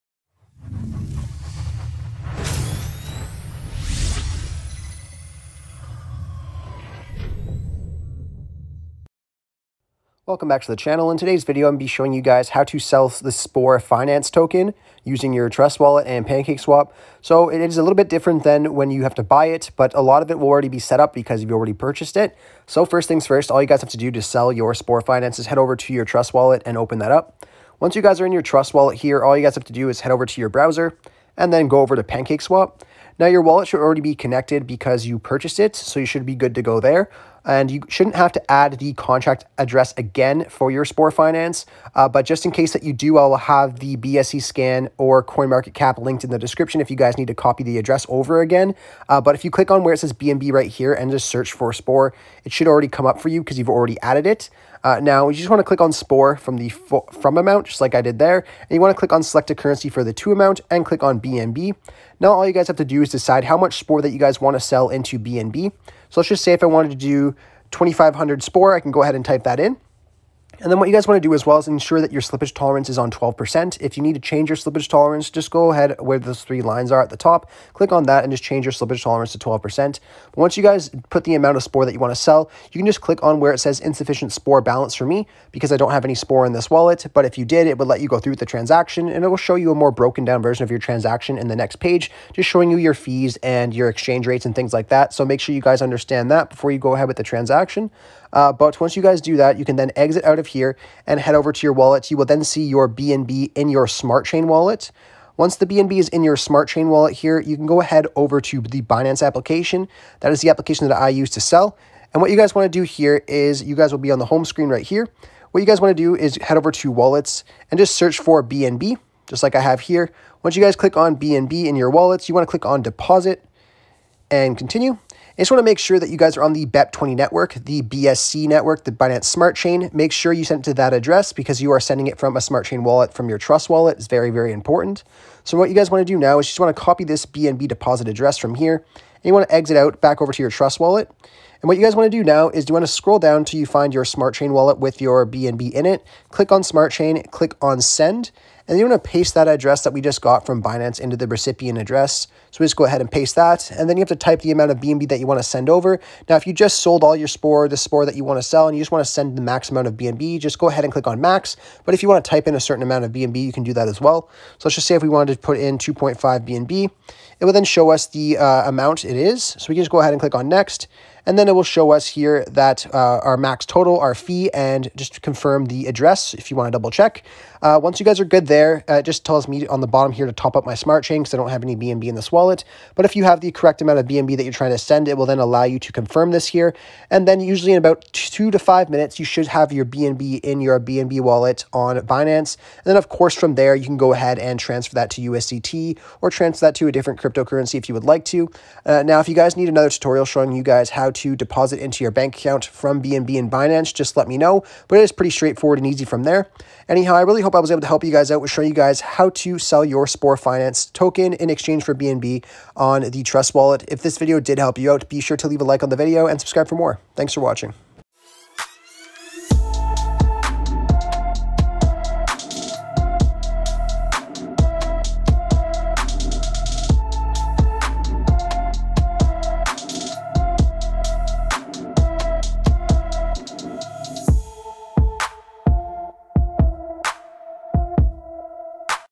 How To Sell Spore Finance On Trust Wallet 💰| How To Sell Spore Finance on Pancakeswap 2021
In today's video we show you How To Sell Spore Finance On Trust Wallet. It's super easy to do takes less then 5 minutes. If you're looking for more Tutorials we have plenty!

If you don't see Spore Finance on Pancakeswap you must copy the contract address from the link below and paste it into your search bar on pancakeswap.